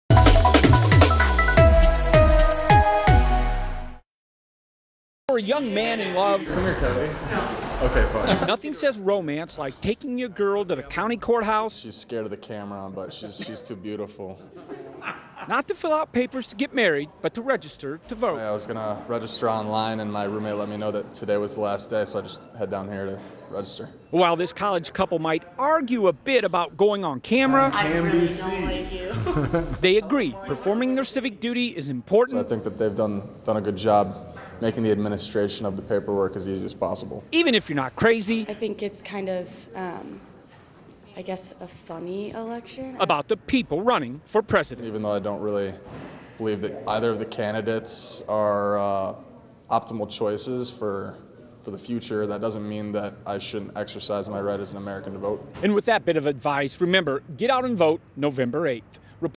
People register to vote before deadline passes in Kansas
People took the time to register to vote Tuesday before the deadline passed in Kansas.